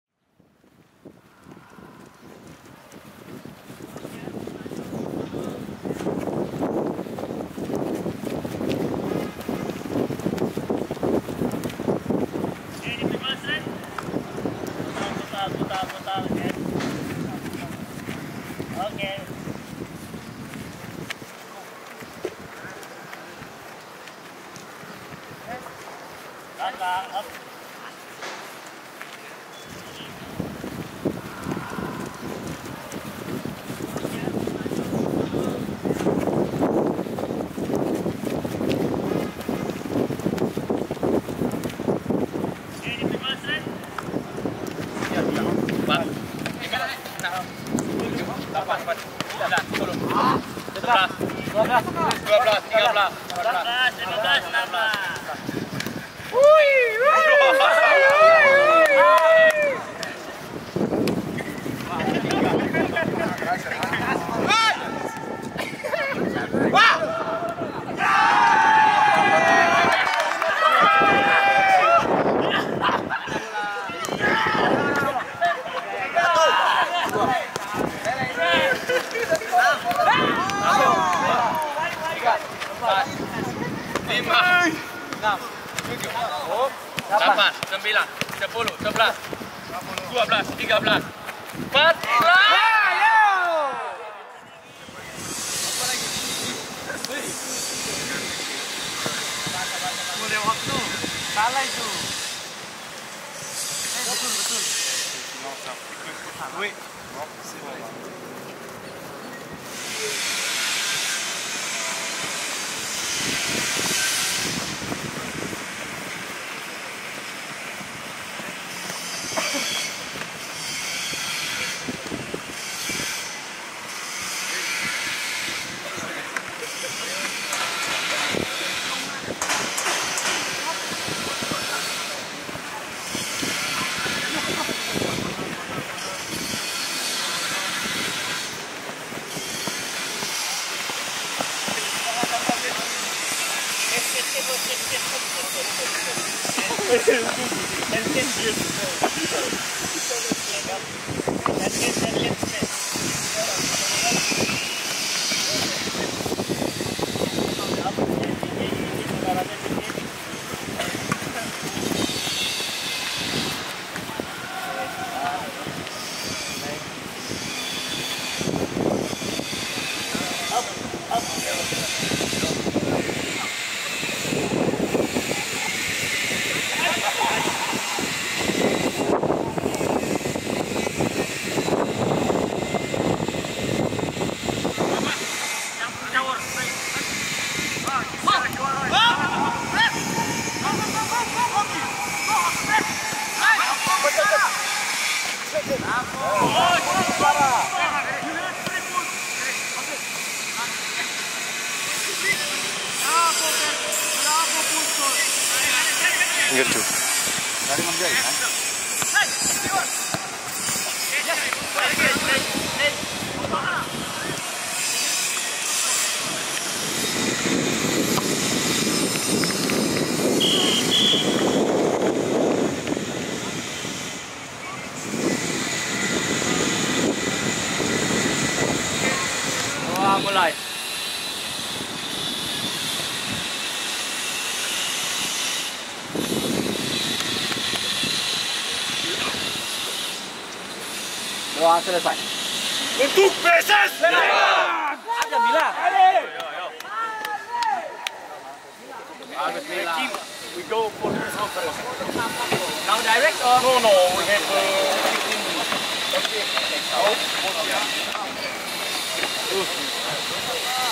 Official Training: Super Elja Incar Poin Penuh Atas Tuan Rumah Persikabo
PSS Fans, simak persiapan terakhir Kim Kurniawan dan rekan-rekan di sesi official training guna laga menghadapi Persikabo 1973, Selasa (8/8) sore di Stadion Wibawa Mukti, Bekasi.

Selengkapnya, saksikan hanya di YouTube PSS TV!